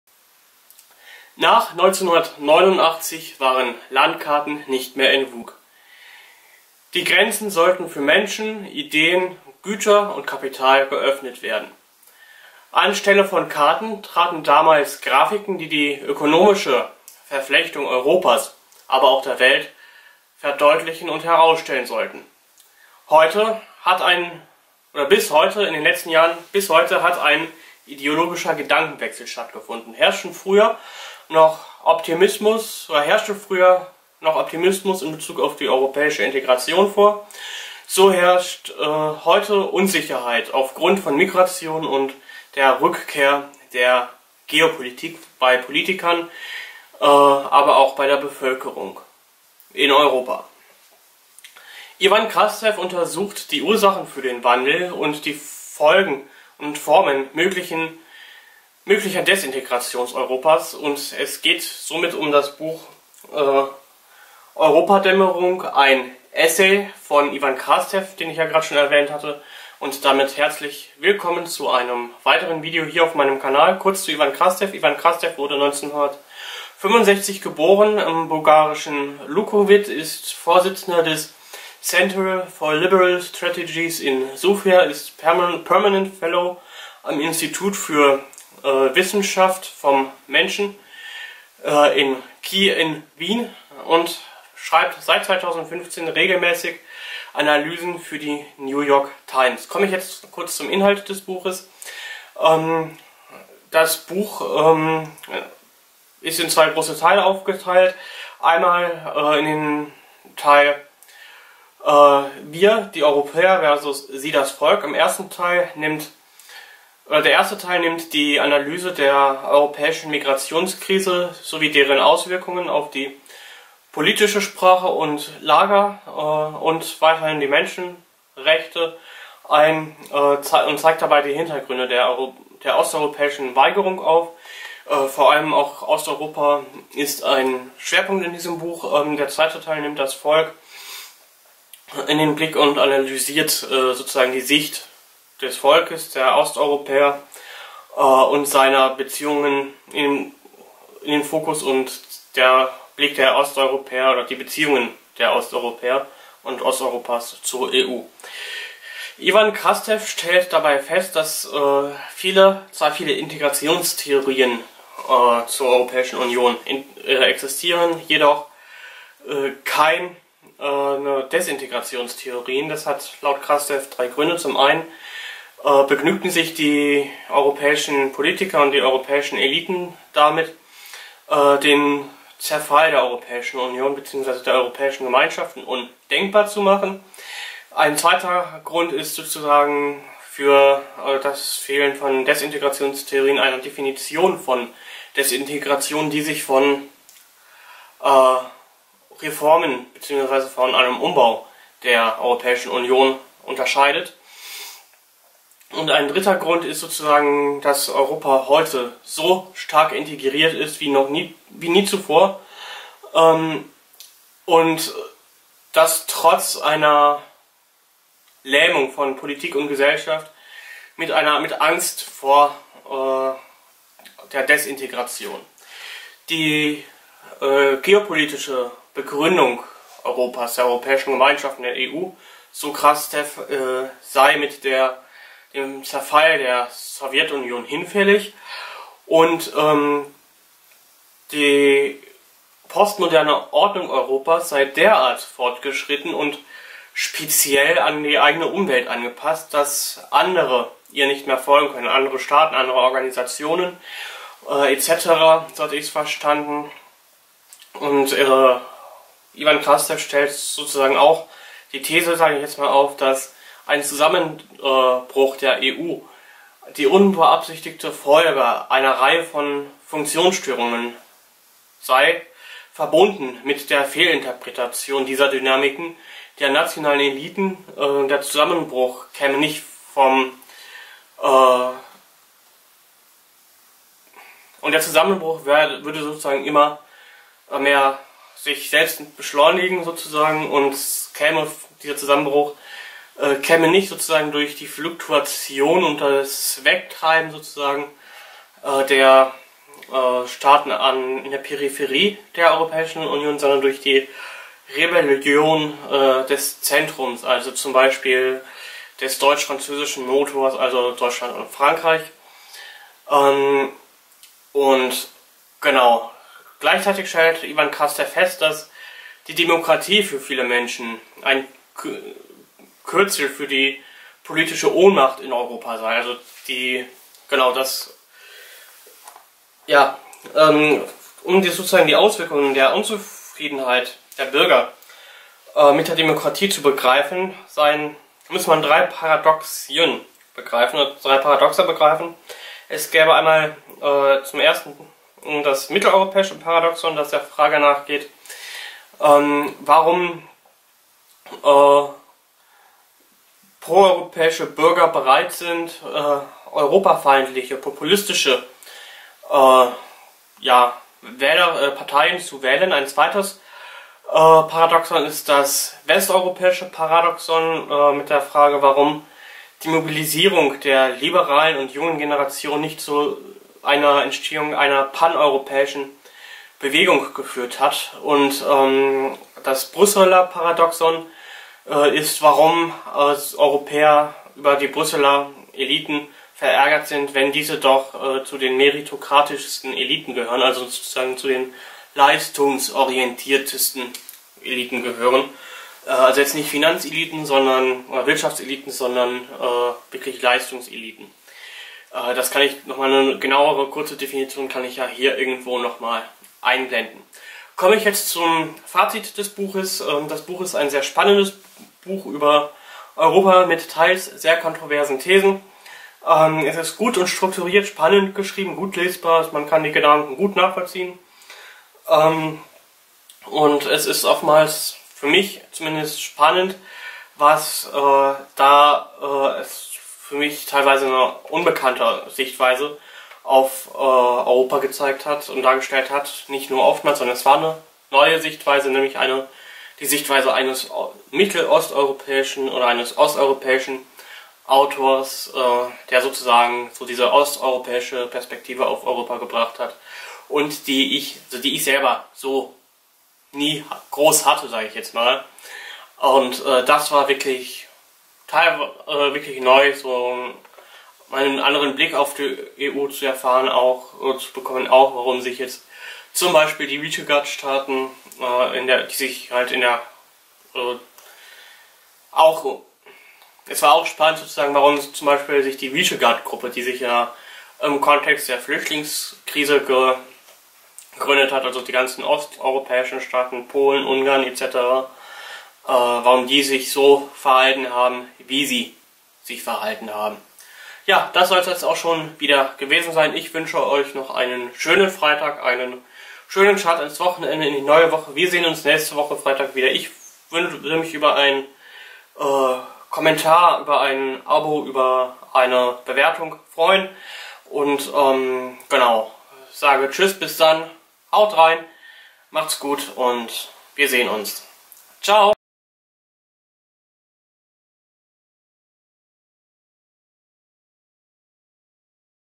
"Europadämmerung - Ein Essay" - Ivan Krastev| Fabian Elbs
Klapentext:

Nach 1989 waren Landkarten plötzlich nicht länger in Mode. Die Grenzen sollten geöffnet werden für Menschen, Güter, Kapital und Ideen. An die Stelle der alten Karten traten Graphiken, welche die ökonomische Verflechtung innerhalb der EU illustrierten. Heute erleben wir einen ideologischen Gezeitenwechsel: Wo die Mehrheit der Europäer noch vor einigen Jahren optimistisch auf die Globalisierung blickte, empfinden sie Migration und die Rückkehr der Geopolitik als Quelle der Unsicherheit. Ivan Krastev untersucht die Ursachen für diesen Wandel und erörtert, welche Formen die europäische Desintegration annehmen könnte. Ein Zerfall der EU, so Krastev, wäre eine Tragödie, die den Kontinent zu internationaler Bedeutungslosigkeit verurteilen würde.

Details:

Ivan KrastevEuropadämmerung - Ein EssayD: 14,00 €A: 14,40 €CH: 20,90 sFrErschienen: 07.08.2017edition suhrkamp 2712, Taschenbuch, 143 SeitenISBN: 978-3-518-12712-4Auch als eBook erhältlich